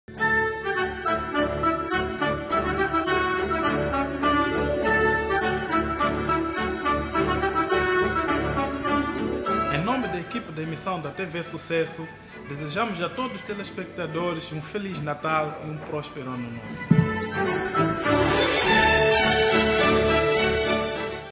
Emissao Tv
Amadeu da Barca deseja a todos um Feliz Natal e um prospero Ano Novo em nome da emissão da TV Sucesso.